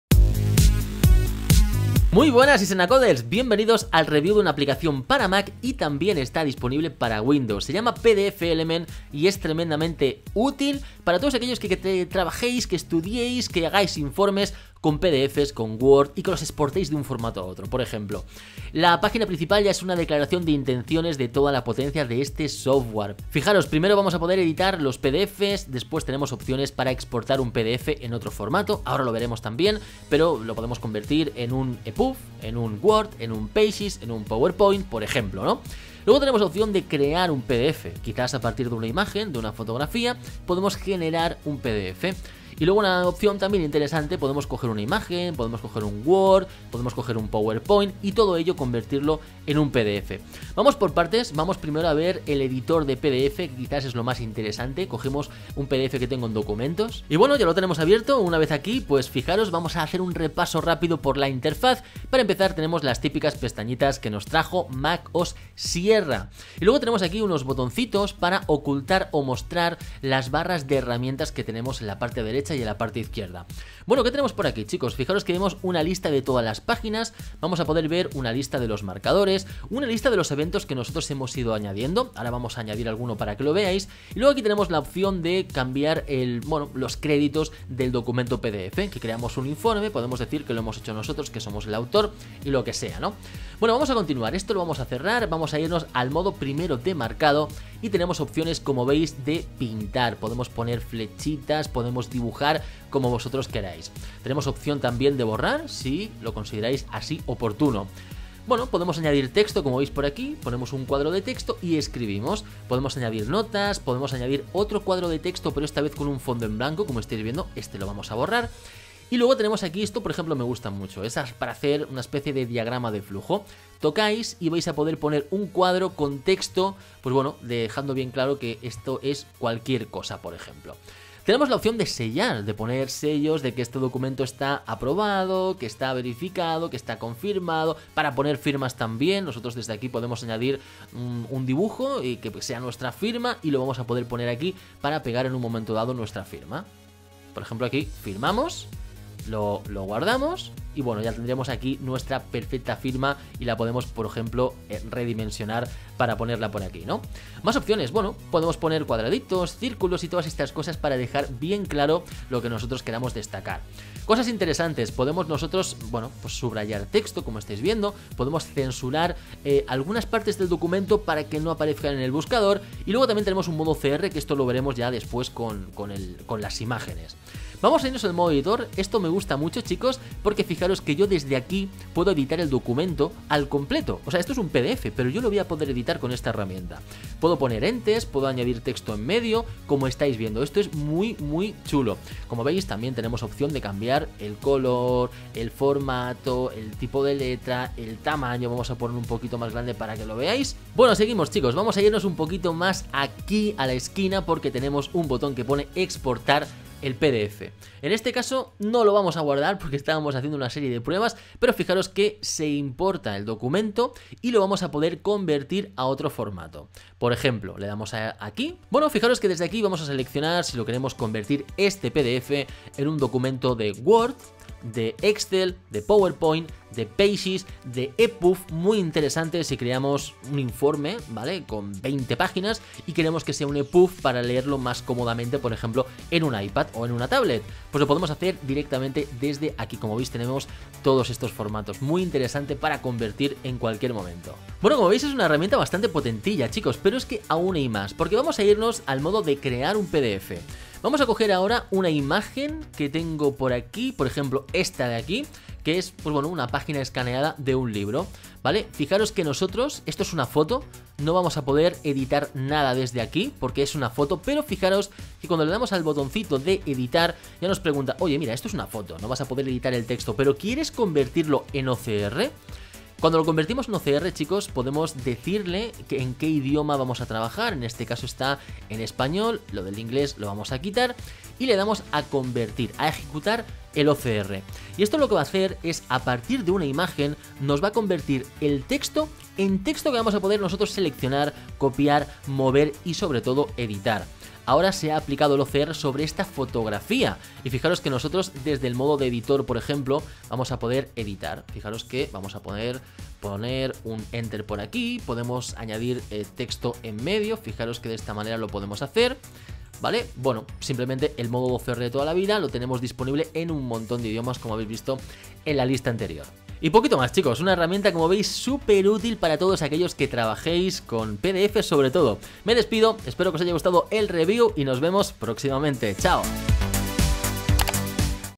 PDFelement, edita tus PDF como un profesional de forma sencilla
PDFelement es una herramienta potentísima para Mac (también disponible en Windows) con la que podemos editar los PDF, añadir figuras, firmas, crear los propios PDF o incluso exportarlos a epub, docx, xlsx y mucho más.

[SÍGUENOS]

[ESCRÍBENOS]

[CRÉDITOS]